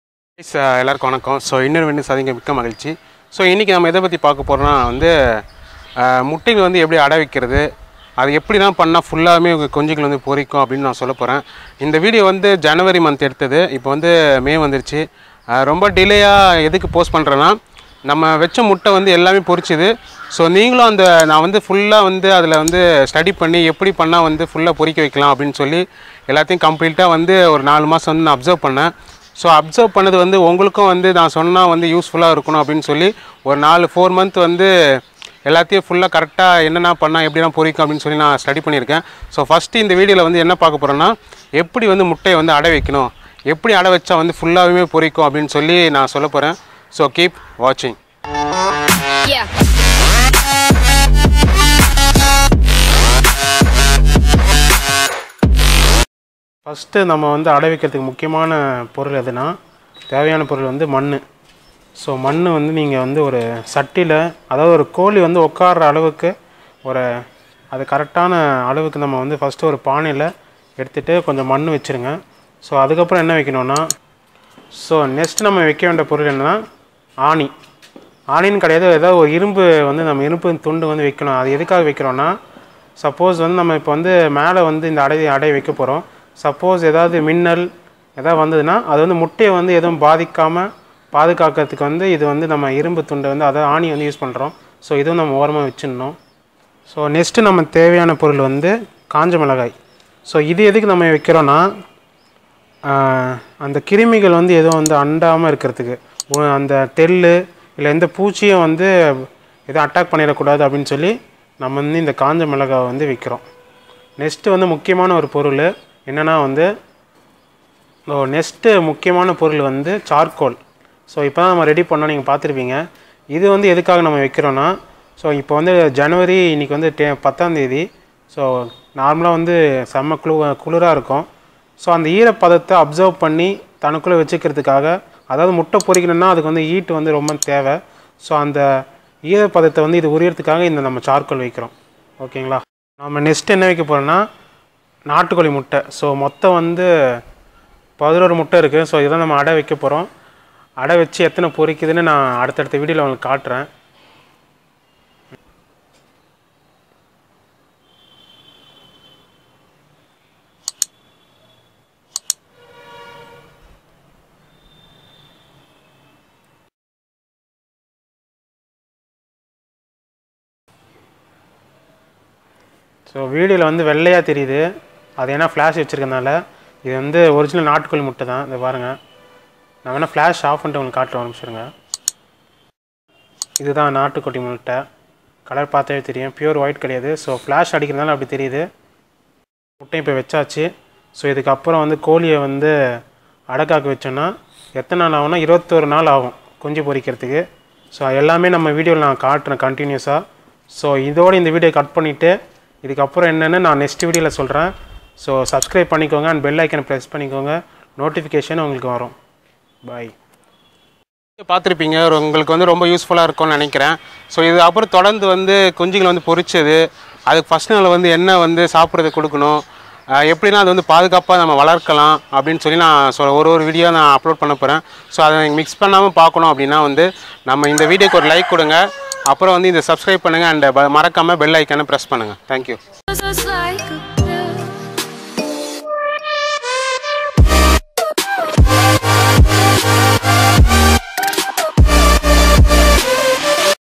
The Success Story Behind Successful Egg Hatching !!!
Hi Friends, In this Video,We are Going to See Useful for all of you.

As all Know, I am living in Chennai ,am owing a Little farm in My house.

Today in This Video , Iam going to tell Successful story of Egg Hatching.

So Watch the Video till END and Share ur Feedback !

My Another Channel  for Weight Loss Tips -